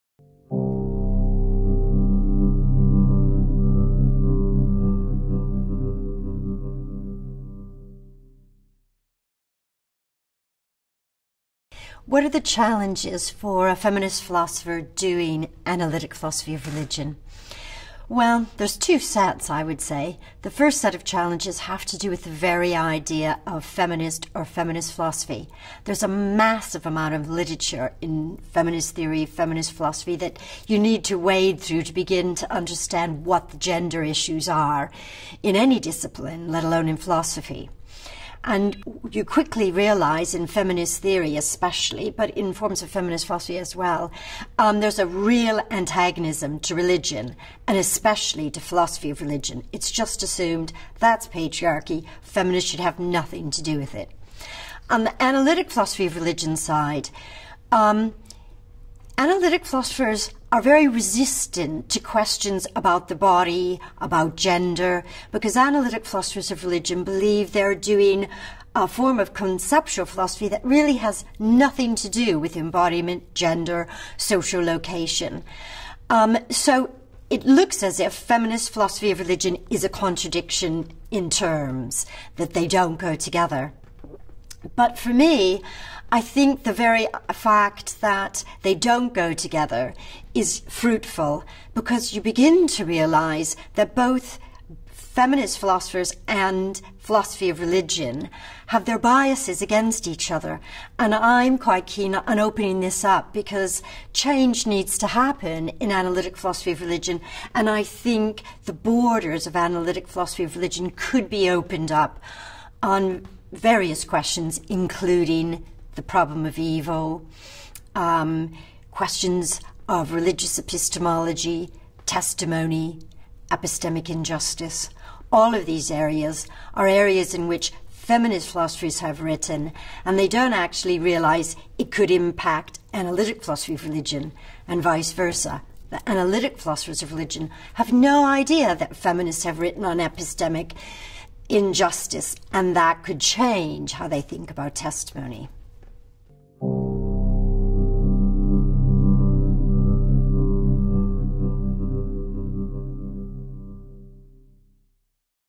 What are the Challenges for a Feminist Philosopher of Religion? (Pamela Sue Anderson)
In this short interview, Pamela Anderson explores some of the difficulties for feminist philosophers of religion doing analytic philosophy.